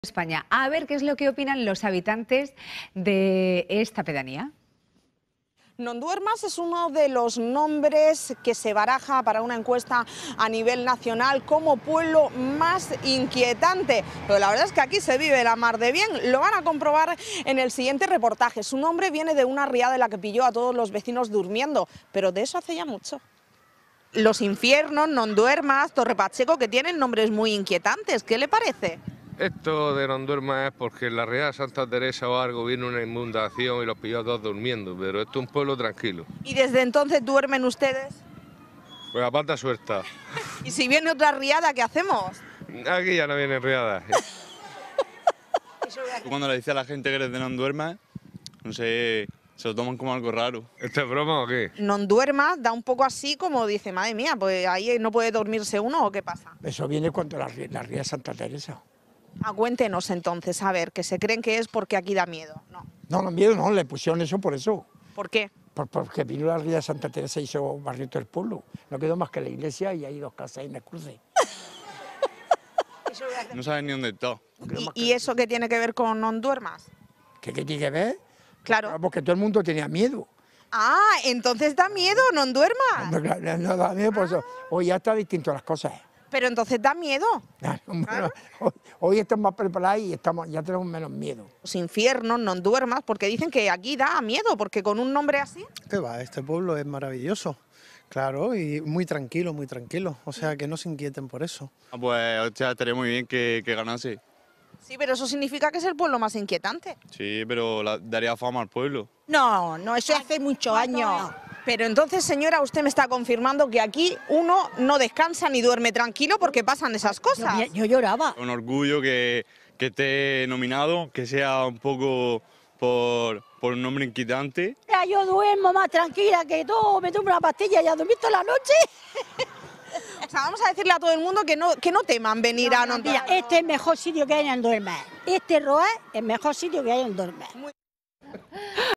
Nonduermas, ¿Un pueblo inquietante?
La pedanía murciana de Nonduermas es candidata a pueblo con nombre más inquietante de España. Pero ¿Qué opinan sus vecinos?